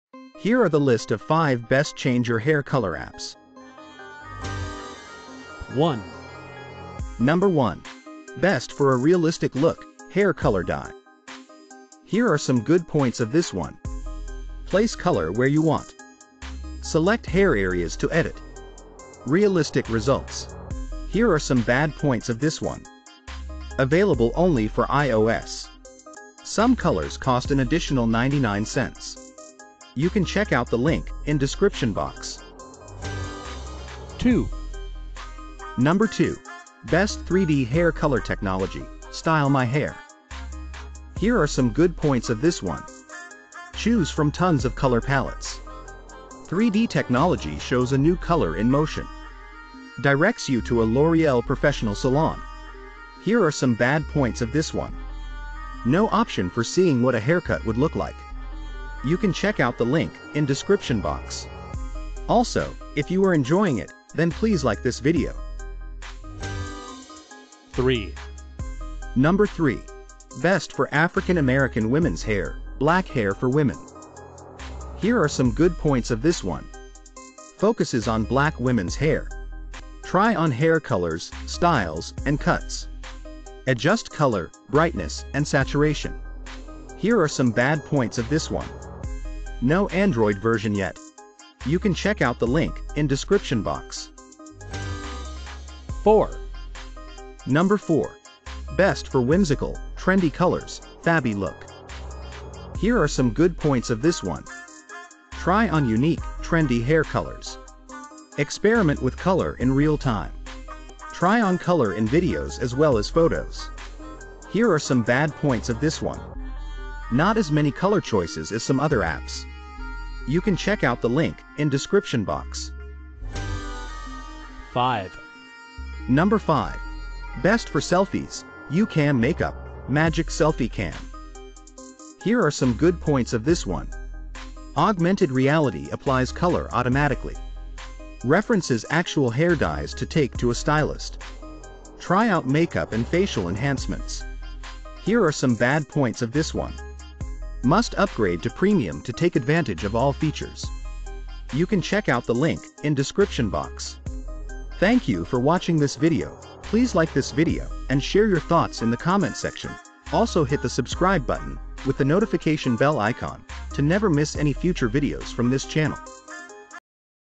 5 Best Change-Your-Hair-Color Apps | For Android and IOS
5 Best Change-Your-Hair-Color Apps | For Android and IOS

Best Change-Your-Hair-Color Apps

01. Best for a Realistic Look- Hair Color Dye

02. Best 3D Hair Color Technology- Style My Hair

03. Best for African American Women's Hair- Black Hair for Women

04. Best for Whimsical- Trendy Colors- Fabby Look

05. Best for Selfies- YouCam Makeup-Magic Selfie Cam

how would i look with grey hair free app